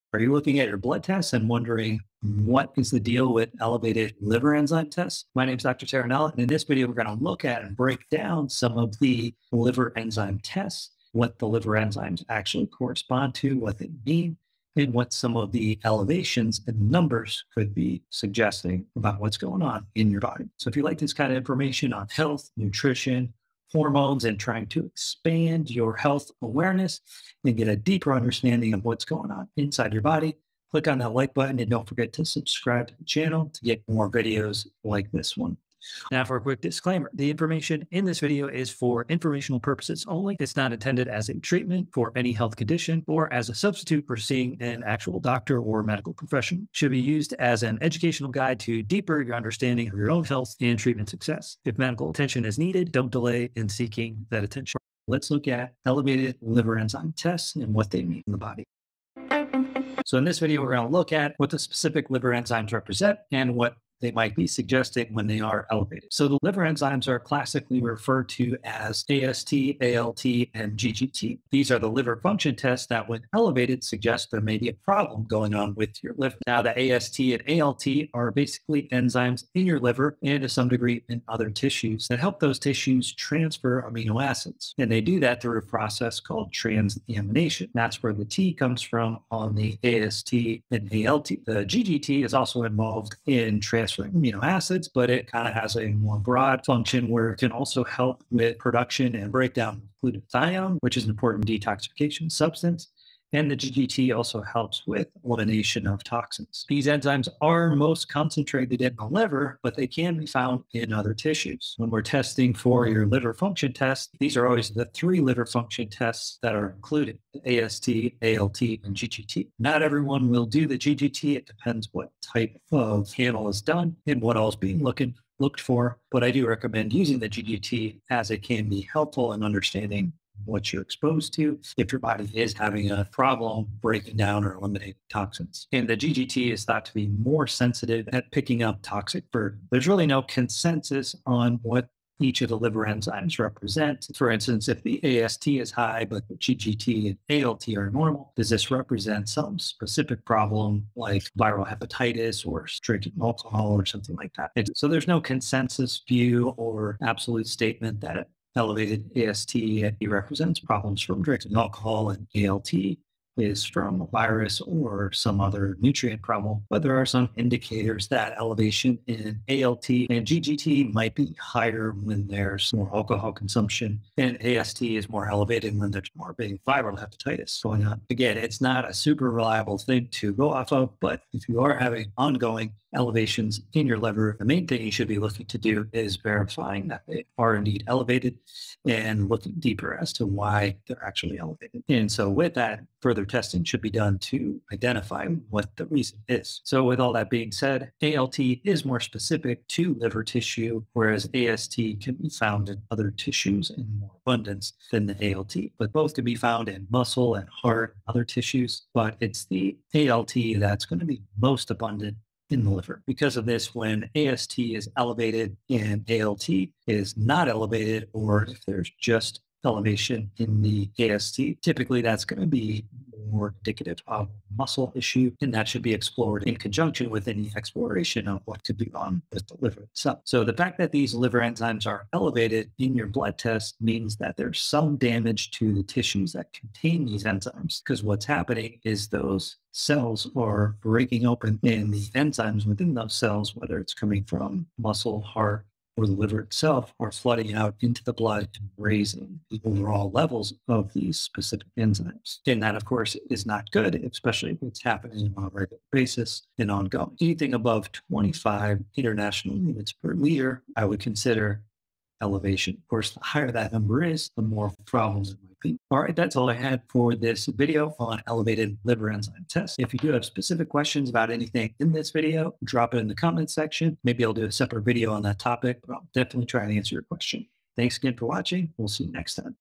Elevated Liver Enzyme Tests Explained
Are you looking at your blood tests and wondering what is the deal with elevated liver enzyme tests?  My name's Dr. Terranella. And in this video, we're gonna look at and break down some of the liver enzyme tests, what the liver enzymes actually correspond to what they mean. And what some of the elevations and the numbers could be suggesting about what's going on in your body.
So in this video, we're gonna look at what the specific liver enzymes represent and what they might be suggesting when they are elevated. So the liver enzymes are classically referred to as AST, ALT and G G T.

These are the liver function tests that when elevated suggests there may be a problem going on with your liver. . Now the AST and alt are basically enzymes in your liver and to some degree in other tissues that help those tissues transfer amino.

And they do that through a process called transamination that's where the T comes from on the asst and alt the G GT is also involved in transferring amino acids, but it kind of has a more broad function where it can also help with production and breakdown of glutathione, which is important detoxification.

And the G G T also helps with elimination of toxins. These enzymes are most concentrated in the liver, but they can be found in other tissues when we're testing for your liver function tests. These are always the three liver function tests that are included.

The asst alt and G G T not everyone will do the G G T. It depends what type of panel is done and what all is being. Looked for, but I do recommend using the G G T as it can be helpful and understanding what you're exposed to. If your body is having a problem breaking down or eliminating toxins. And the GT is thought to be more sensitive at picking up toxic burden.

There's really no consensus on what. Each of the liver enzymes represent, for instance, if the AST is high, but the GT and alt are normal, does this represent some specific problem, like viral hepatitis or drinking alcohol or something like that?

And, so there's no consensus view or absolute statement that elevated AST represents problems from drinking, alcohol and alt. Is from a virus or some other nutrient problem, but there are some indicators that elevation in alt and G GT might be higher when there's more alcohol consumption and AST is more elevated when there's more a viral hepatitis going on again, it's not a super reliable thing to go off of, but if you are having ongoing.

Elevations in your liver. The main thing you should be looking to do is verifying that they are indeed elevated and looking deeper as to why they're actually elevated. And so with that further testing should be done to identify what the reason is. So with all that being. Alt is more specific to liver tissue. Whereas AST can be found in other tissues in more abundance than the alt but both can be found in muscle and heart and other tissues, but it's the alt that's gonna be most abundant. In the liver because of this, when AST is elevated in alt it is not elevated.

Or if there's just elevation in the AST, typically that's gonna be more indicative of a muscle issue. And that should be explored in conjunction with any exploration of what could be wrong with the liver itself. So the fact that these liver enzymes are elevated in your blood test means that there's some damage to the tissues that contain these enzymes.

Cause what's happening is. Cells are breaking open in the enzymes within those cells, whether it's coming from muscle heart or the liver itself are flooding out into the blood and raising the overall levels of these specific enzymes. And that of course is not good, especially if it's happening on a regular basis and ongoing anything above 25 international units per leader, I would consider.

Elevation, of course the higher, that number is the more  problems it might be.